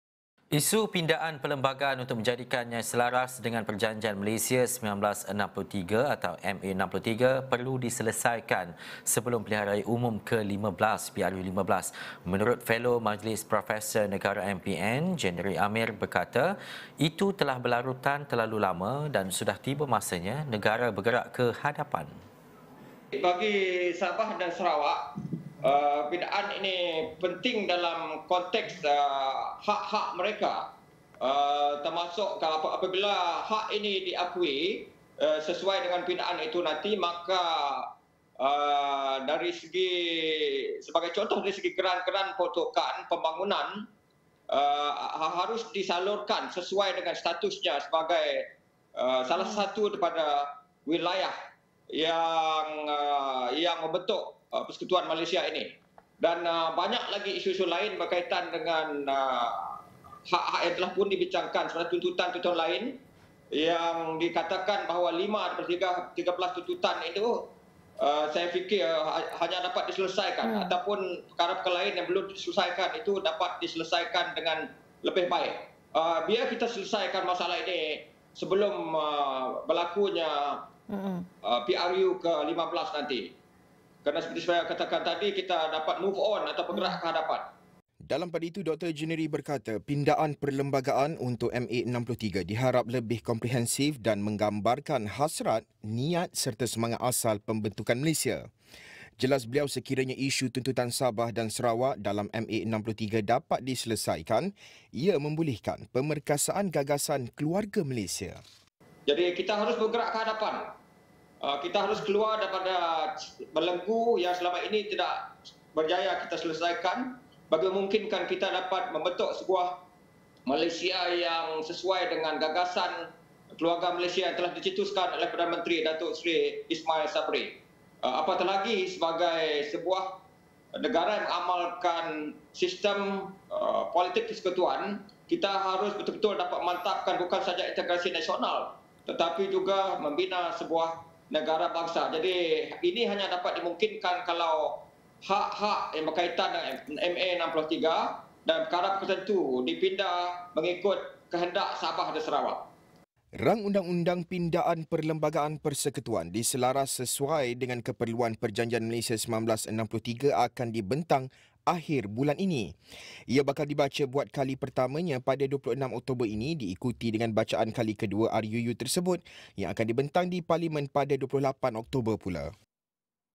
Pinda Perlembagaan | Selesaikan tuntutan MA63 sebelum PRU15
Isu pindaan perlembagaan untuk menjadikannya selaras dengan Perjanjian Malaysia 1963 (MA63) perlu diselesaikan sebelum Pilihan Raya Umum ke-15 (PRU15).

Menurut Felo Majlis Profesor Negara (MPN), Jeniri Amir, perkara itu telah berlarutan terlalu lama dan sudah tiba masanya negara bergerak ke hadapan.